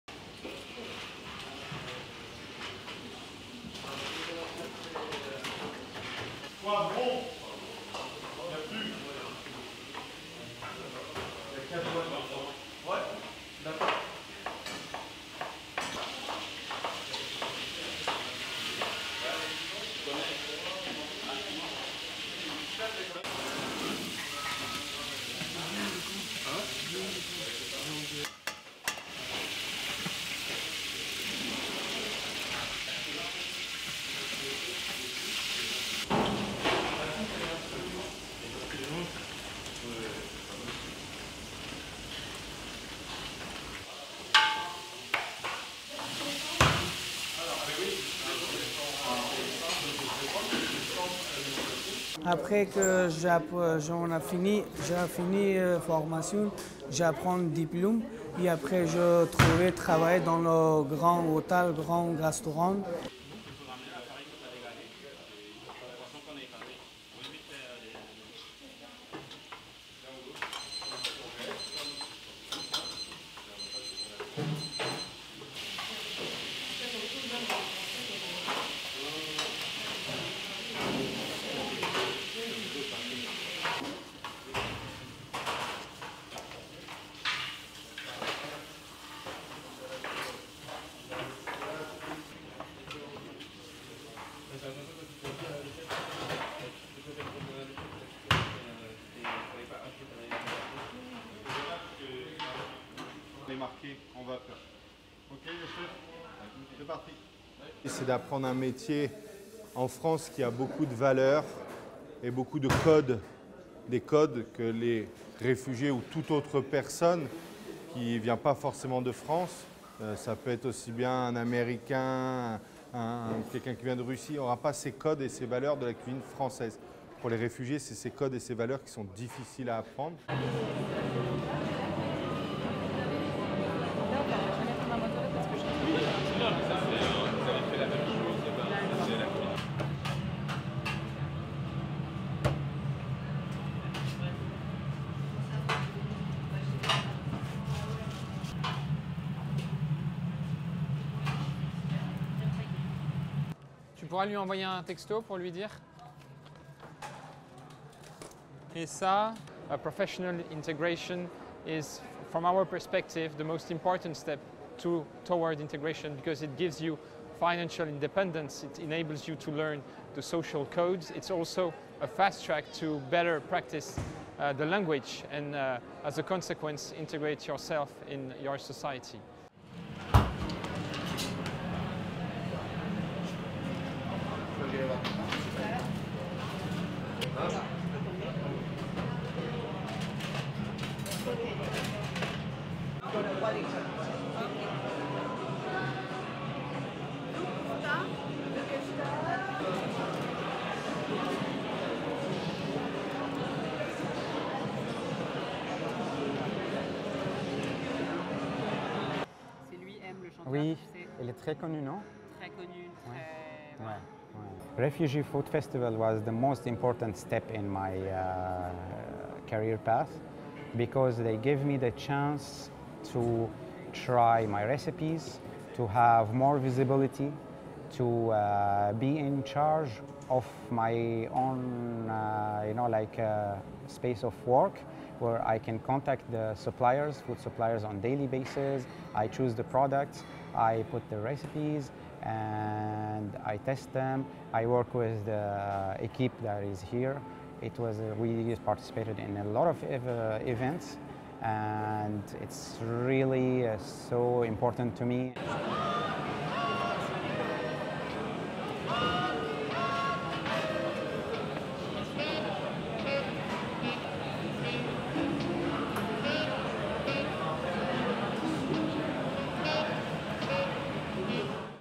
The Associated Press - Refugees in France find home, warmth in cooking
Refugees in France learn to master dishes down to the finest of details. While learning what appeals to the French taste palette, the refugees are also being integrated into society.